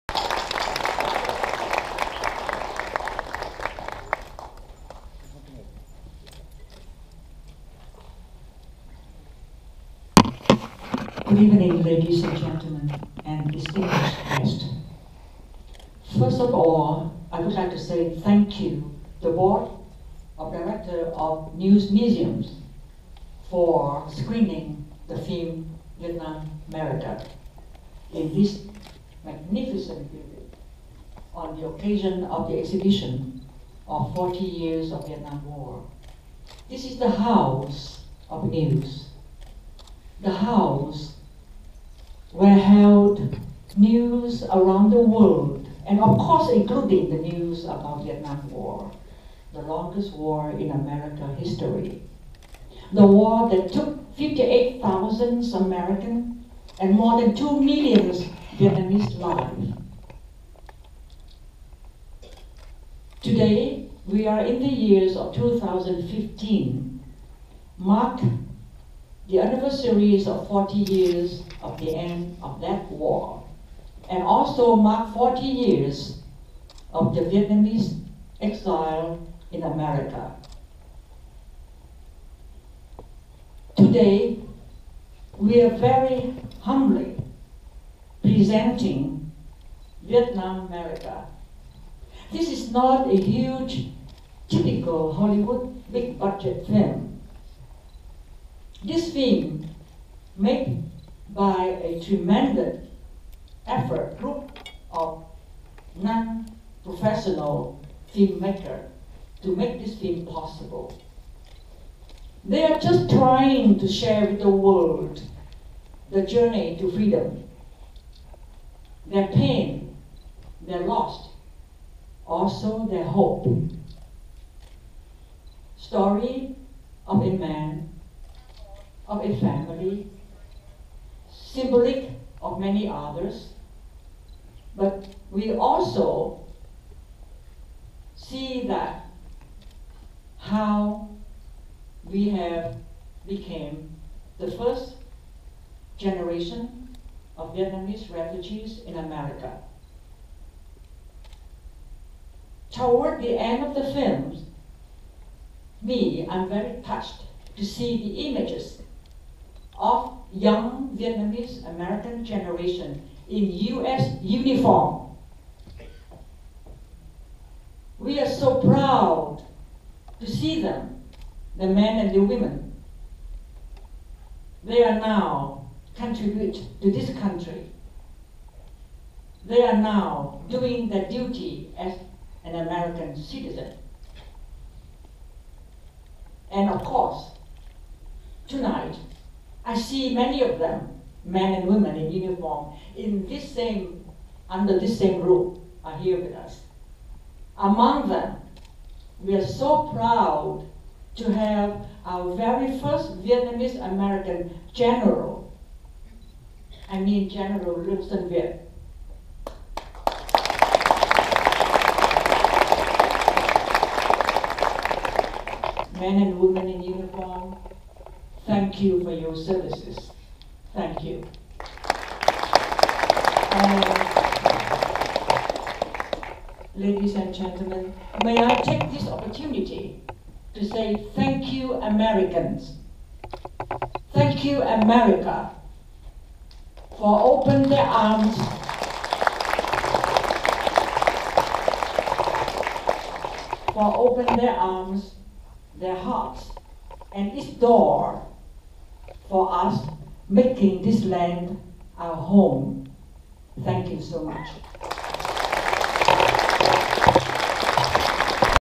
Welcome remark from Legendary Actress Kieu Chinh
Welcome remark from Legendary Actress Kieu Chinh during private screening Vietnamerica at Newseum in Washington D.C. (10/17/15) ... Recording by GoPro Hero 4 Black Edition ... 4k Widescreen Superview.